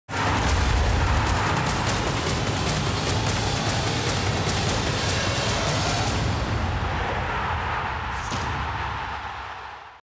6 More Days, Then The Barclays Premier League Is Back!
The assistant referee has raised the board...it's 6! 6 more days until kick-off in the Barclays Premier League!